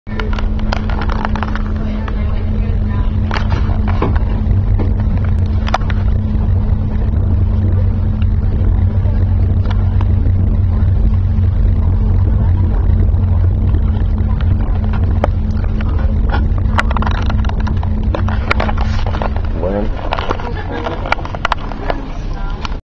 Alps 2
Our Train from Munich to Ljubljana went through the Austrian Alps.